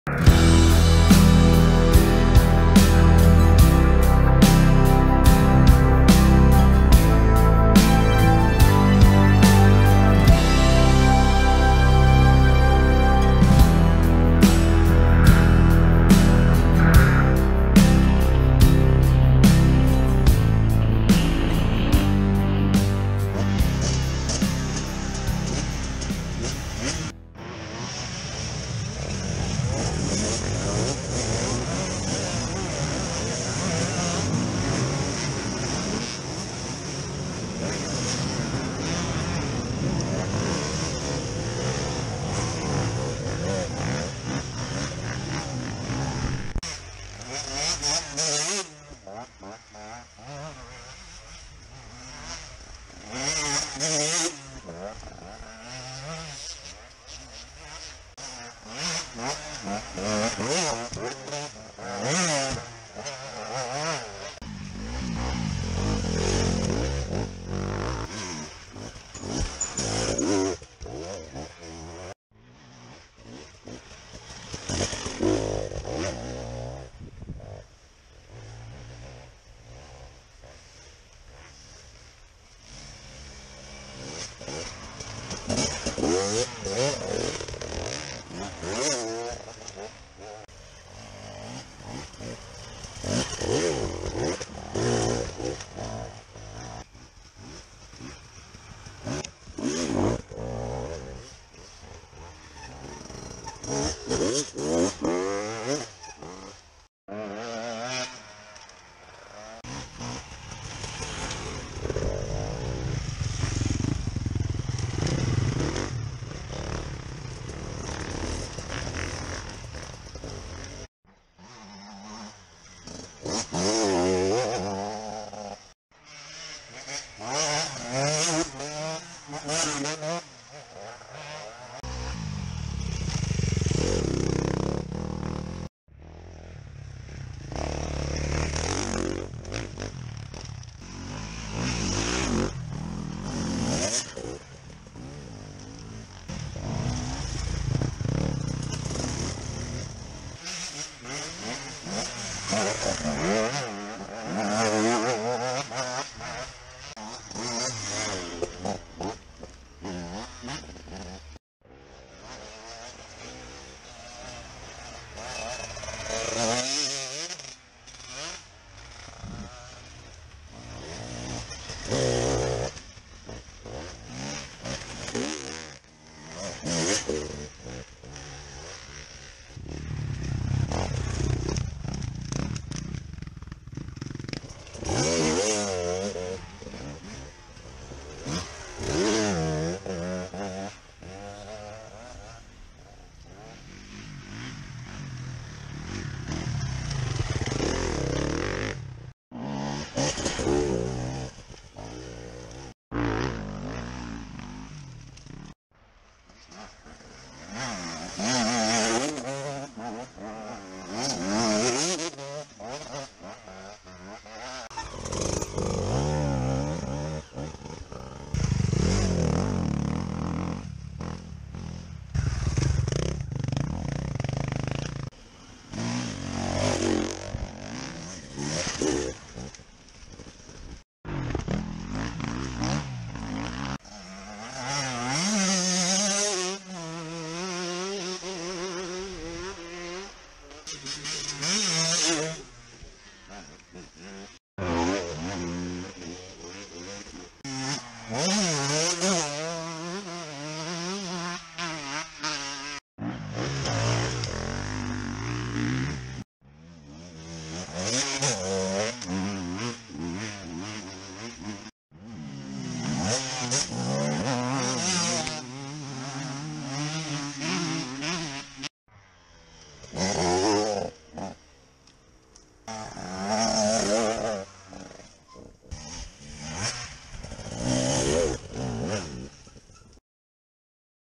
JSMK 2h sarjaenduro
Sunnuntain Sarjaendurot hieman sateisessa kelissä.